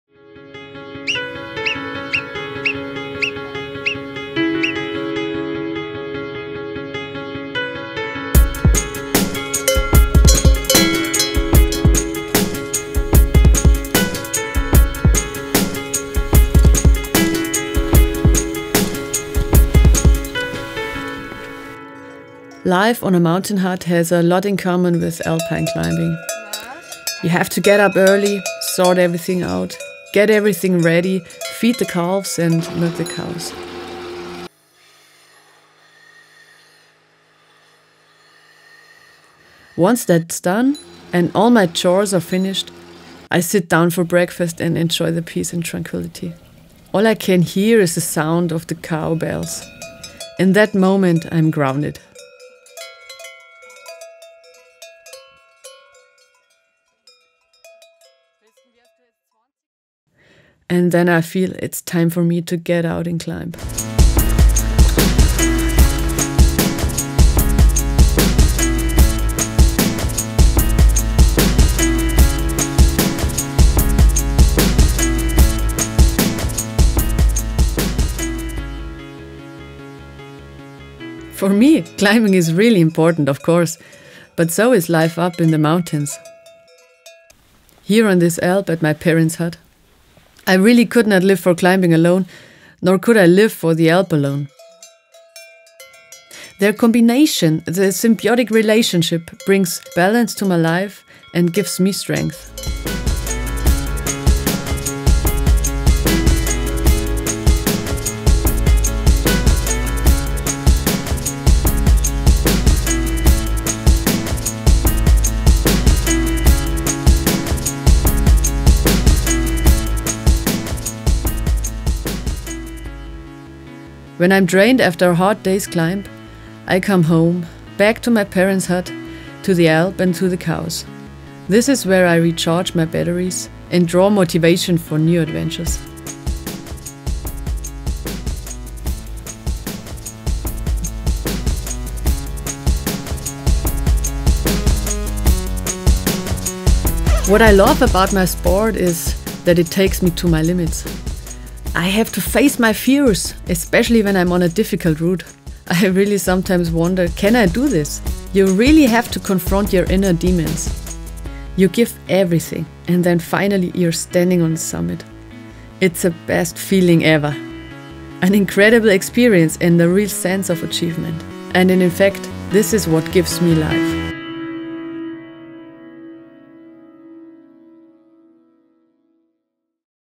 What gives you life? - PRO Story Nina Schlesener
Marmot PRO Christina “Nina” Schlesener (31) has lived her life in the vertical arena ever since she was born. Even before she started climbing she dreamt of living in and with the mountains. Nina grew up in the Bavarian Alps, with the famous Watzmann in her backyard. One thing always was clear to her: She simply had to be a mountain guide! In 2010, she began her guide’s training which she successfully finished in 2012 as a fully qualified mountain guide – the youngest female in Germany by far. With it realized a childhood dream.